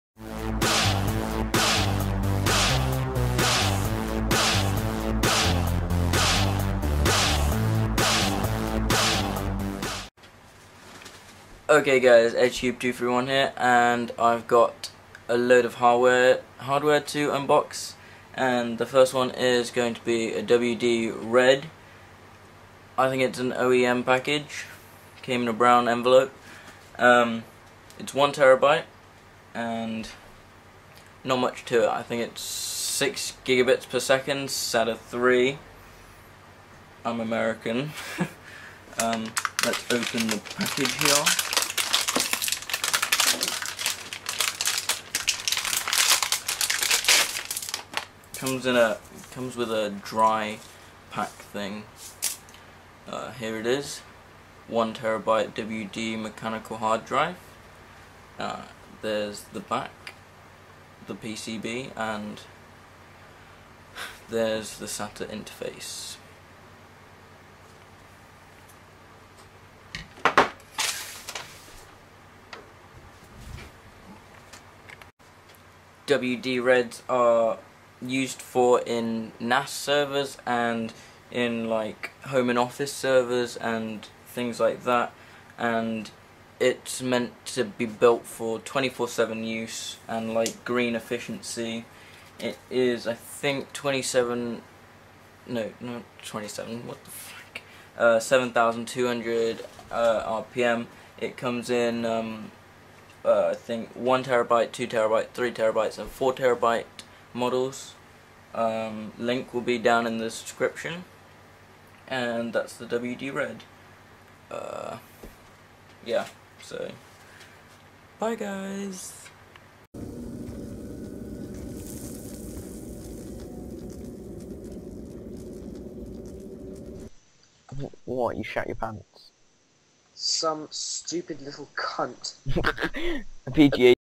Western Digital Red NASware 1tb | Unboxing and First look #001 | TechEdgey231
I have a look at the hardware of the 1 tb mechanical wd red hard drive. bear in mind i am a novice unboxer and not any sort of linus pro Unboxer!
WD Red hard drives are designed and tested for compatibility in the unique 24x7 operating environment and demanding system requirements of home and small office NAS.
Western Digital claims that these tweaks result in a 35 per cent MTBF (mean time between failures) improvement over standard desktop drives, and is quick to point out that the optimisation ultimately reduces the long-term cost of NAS ownership.